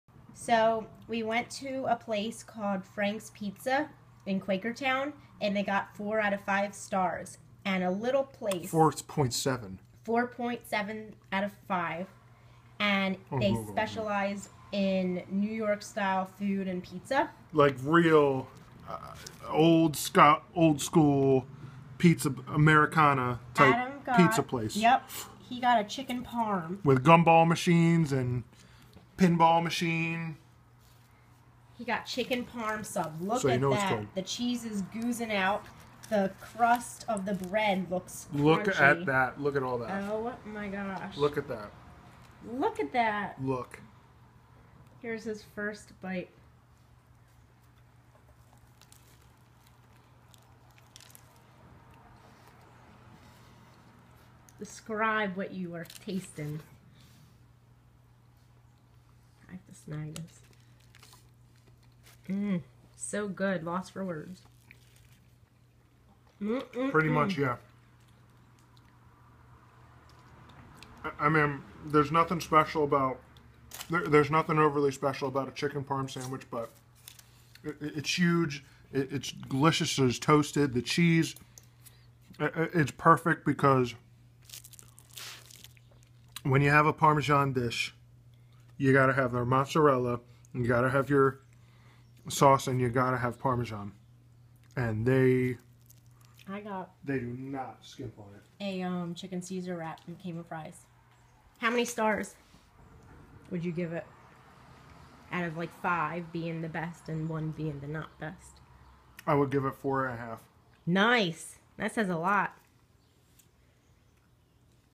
Food review! chicken parm edition!
Adam reviewed a chicken parmesan sandwich from "Franks Pizza" and it was delicious!! 5 out of 5 stars!!